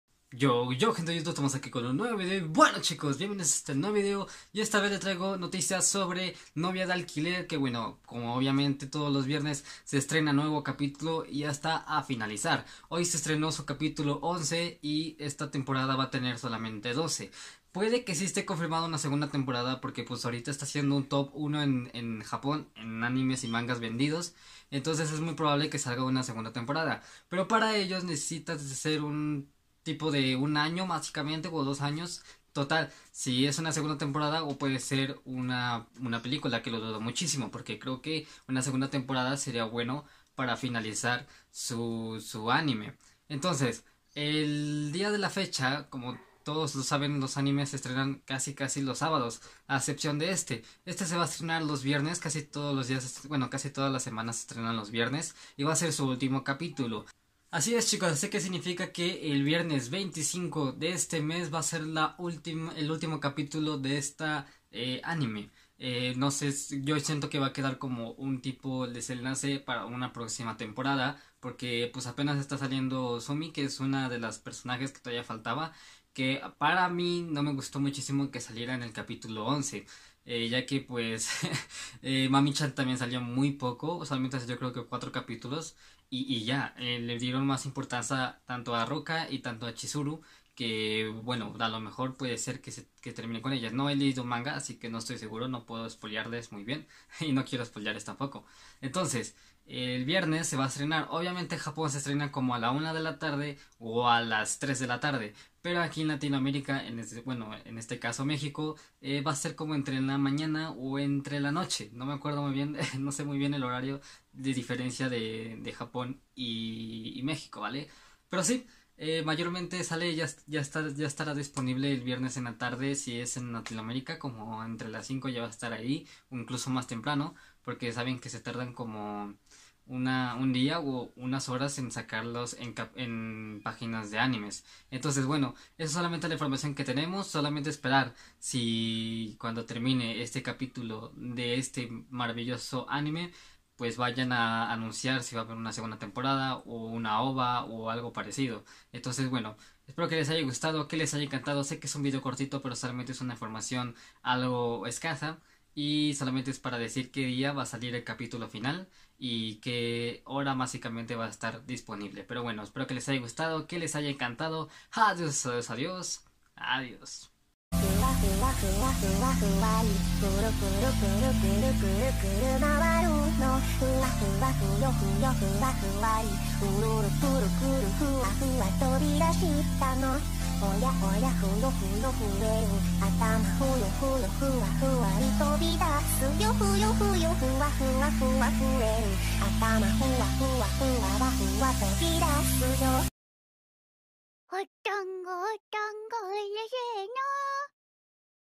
Kanojo, okarishimasu Capítulo 12 | Capítulo Final | Fecha de lanzamiento | Novia de Alquiler
Hola, hoy traigo nuevas noticias sobre Kanojo Okarishimasu y su posible temporada 2.

► No hay descarga...